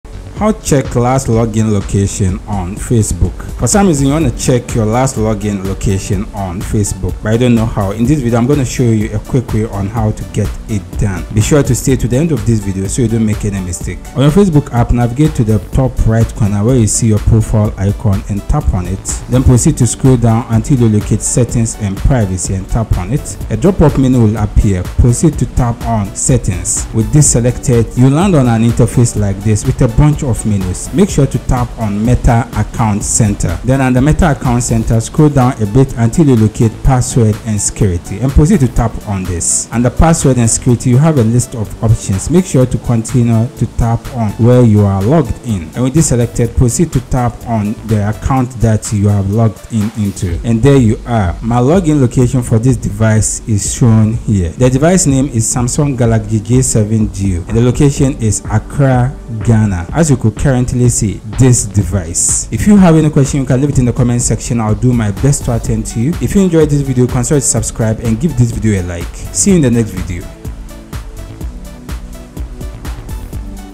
How to Check Last Login Location on Facebook [UPDATE] - Full Guide
Learn How to Check Last Login Location on Facebook with this simple and easy steps.